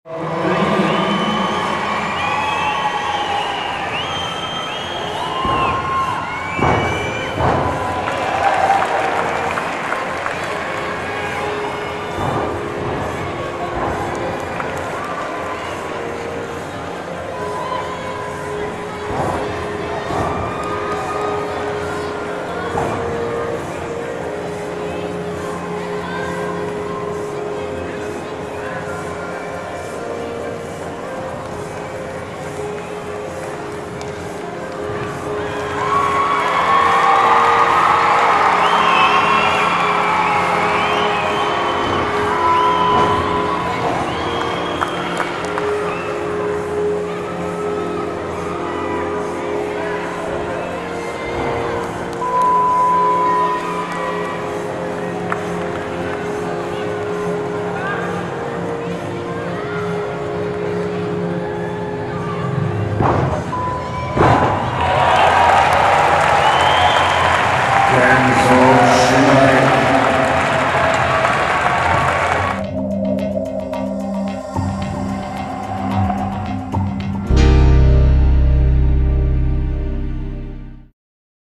Kenzo SHIRAI (JPN) - 2013 Artistic Worlds
FIG Official -- 2013 Artistic Worlds, Antwerp (BEL). Kenzo Shirai (JPN), qualifications, Floor : 16.233 (D score : 7.40, E score : 8.833). Just fantastic !... A new star's born.